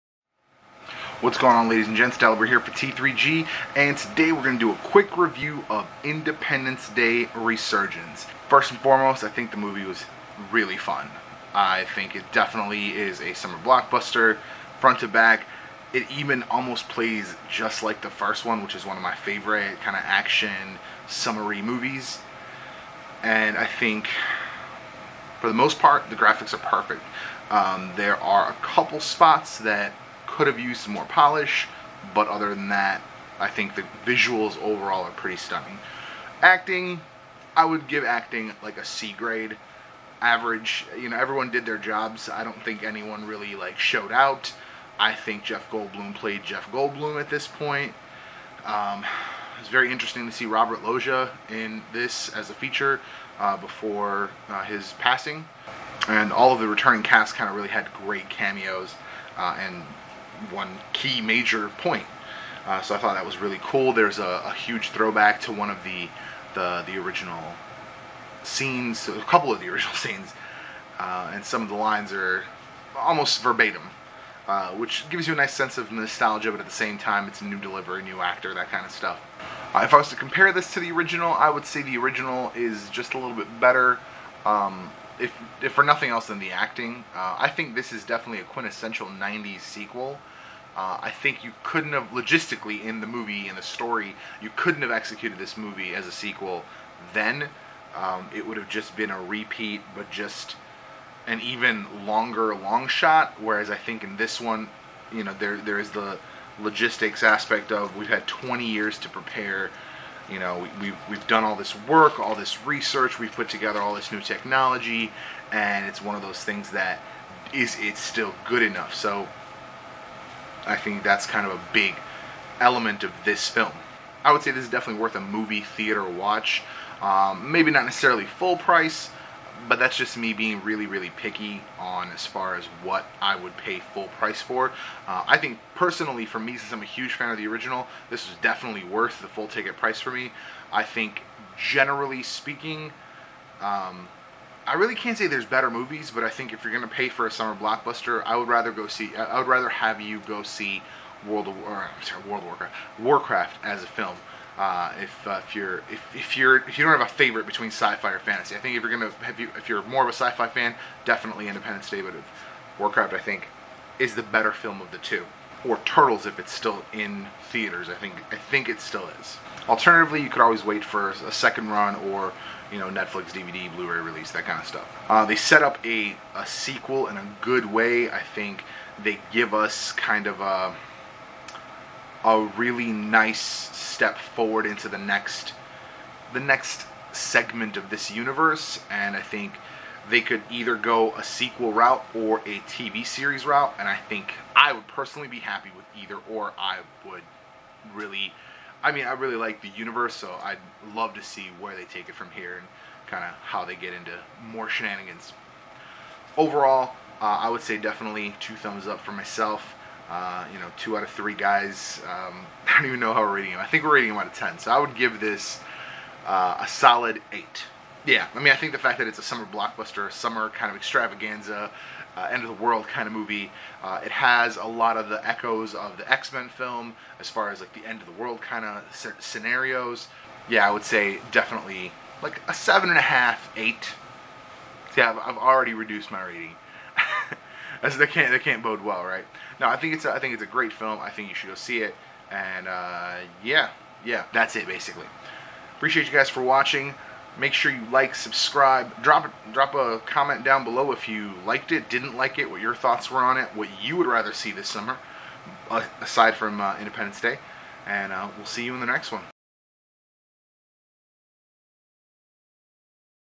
Independence Day Resurgence Non-Spoiler Review - T3G Talks
Dalibor reviews Independence Day Resurgence

T3G Social:

Beme: Those3Guys